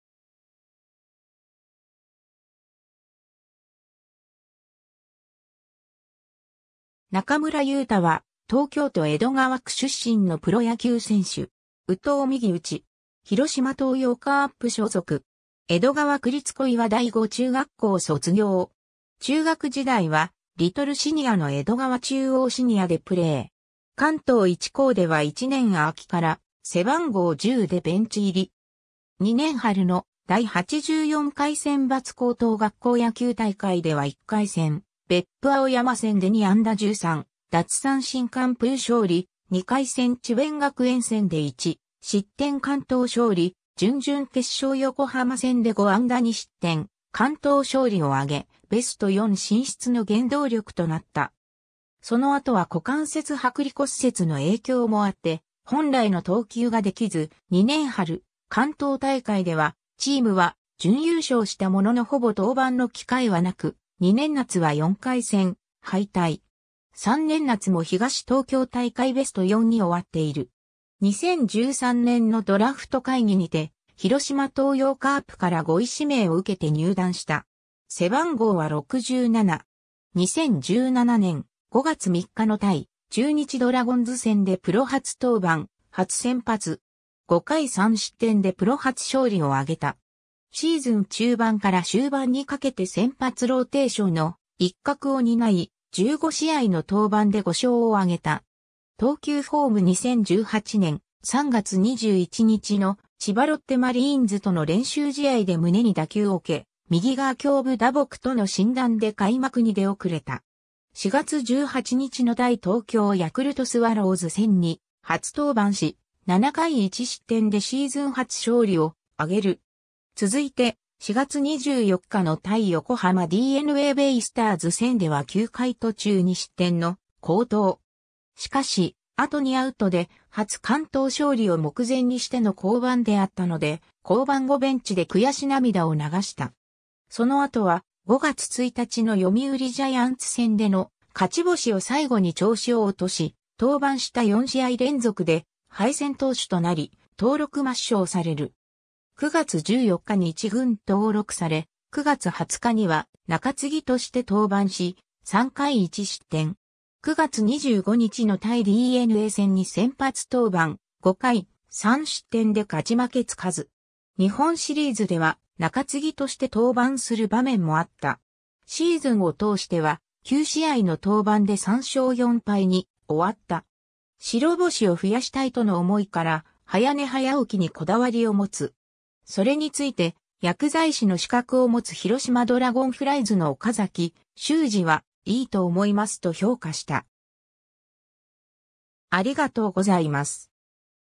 中村祐太
中村 祐太（なかむら ゆうた、1995年8月31日 - ）は、東京都江戸川区出身のプロ野球選手（投手）。
右投右打。
広島東洋カープ所属。
 江戸川区立小岩第五中学校卒業。
中学時代はリトルシニアの江戸川中央シニアでプレイ。
関東一高では1年秋から背番号10でベンチ入り。
2年春の第84回選抜高等学校野球大会では1回戦別府青山戦で2安打13奪三振完封勝利、2回戦智弁学園戦で1失点完投勝利、準々決勝横浜戦で5安打2失点完投勝利をあげ、ベスト4進出の原動力となった。
（準決勝で田村龍弘、北條史也を擁する光星学院に敗退。）その後は股関節剥離骨折の影響もあって本来の投球ができず、2年春関東大会ではチームは準優勝したもののほぼ登板の機会はなく、2年夏は4回戦敗退。
3年夏も東東京大会ベスト4に終わっている。
 2013年のドラフト会議にて、広島東洋カープから5位指名を受けて入団した。
 2017年、5月3日の対中日ドラゴンズ戦でプロ初登板・初先発。
5回3失点でプロ初勝利を挙げた。
シーズン中盤から終盤にかけて先発ローテーションの一角を担い、15試合の登板で5勝を挙げた。
 投球フォーム(2017/11/7 天福球場) 2018年、3月21日の千葉ロッテマリーンズとの練習試合で胸に打球を受け、「右側胸部打撲」との診断で開幕に出遅れた。
4月18日の対東京ヤクルトスワローズ戦に初登板し、7回1失点でシーズン初勝利を挙げる。
続いて、4月24日の対横浜DeNAベイスターズ戦では9回途中2失点の好投。
しかし、あと2アウトで初完投勝利を目前にしての降板であったので、降板後ベンチで悔し涙を流した。
その後は、5月1日の読売ジャイアンツ戦での勝ち星を最後に調子を落とし、登板した4試合連続で敗戦投手となり、登録抹消される。
9月14日に1軍登録され、9月20日には、中継ぎとして登板し、3回1失点。
9月25日の対DeNA戦に先発登板、5回3失点で勝ち負けつかず。
日本シリーズでは、中継ぎとして登板する場面もあった。
シーズンを通しては、9試合の登板で3勝4敗に終わった。
 「白星を増やしたい」との想いから、早寝早起きにこだわりを持つ。
それについて薬剤師の資格を持つ広島ドラゴンフライズの岡崎修司は「いいと思います」と評価した。